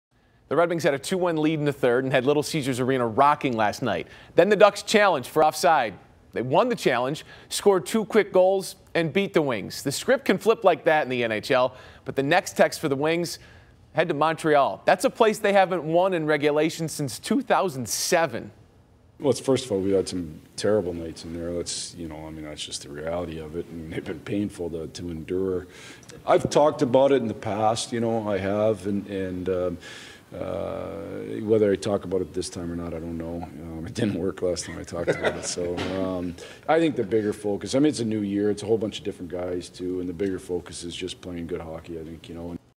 Red Wings head to Montreal, where they haven't won in regulation since 2007
Red Wings head to Montreal, where they haven't won in regulation since 2007. Brad Galli has more.